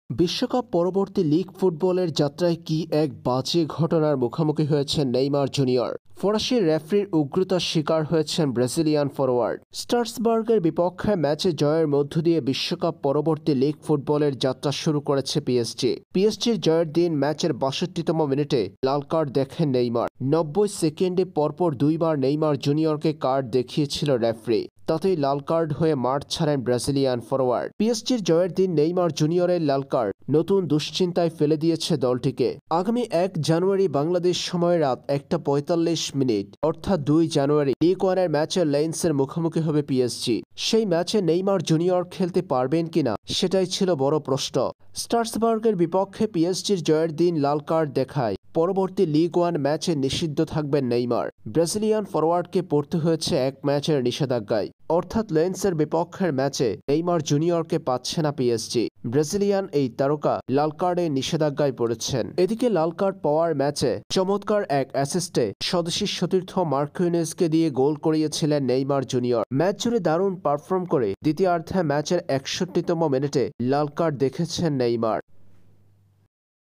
লাল কার্ডের ঘটনার পর এবার নিষিদ্ধ হলেন নেইমার❗কয় ম্যাচের নিষেধাজ্ঞায় নেইমার? জানুন | Neymar Red Card
Neymar news | Neymar Red Card vs Strasbourg | PSG Match | PSG vs Strasbourg 2-1 Match Ligue 1. Neymar banned for 1 match

Messi Neymar Mbappe PSG - নেইমারের লাল কার্ড।  লিগ ম্যাচে লাল কার্ড দেখলেন নেইমার।